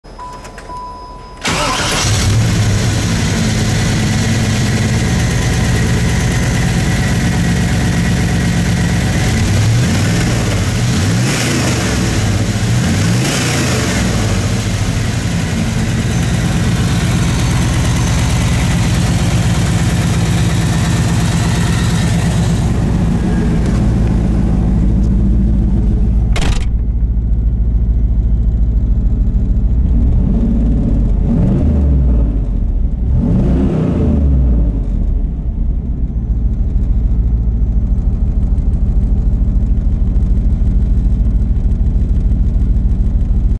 1993 Camaro Z28 LT1 5.7L V8 Complete Engine Motor 186K Miles ENGINE ONLY
***FOR SALE***
1993 Camaro Z28 LT1 5.7L V8 Complete Engine Motor 186K Miles ENGINE ONLY

What's Included:
COMPLETE 1993 LT1 5.7L 350CI ENGINE (INTAKE TO OIL PAN)
ENGINE WIRING HARNESS
ECM COMPUTER
COMPLETE FRONT ACCESSORIES W/ BRACKETS AND BOLTS
MASS AIR FLOW
EXHAUST MANIFOLDS
STARTER